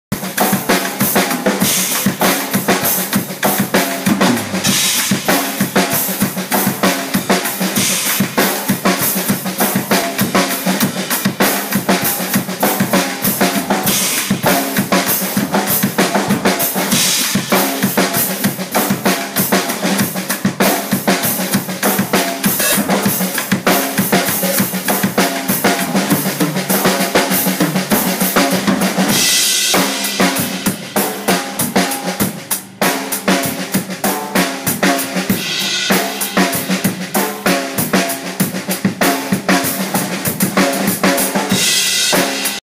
Drum Lesson: 5/4 & 5/8 together.
Here is another example of odd time exercises. 5/4 & 5/8 together. Jon (drum kit in back) plays in 5/4 (Kick 1,3, 5, snare 2, 4)  while Jimmy ( nearest drummer) plays a 5/8. His fills are in 5/16    (RLRLL 4 times) This equals 2 bars of 5/8 or 1 bar of 5/4… depending on your point of view. These drum duets were inspired by the ground breaking work & lessons of Maestro Efrain Toro.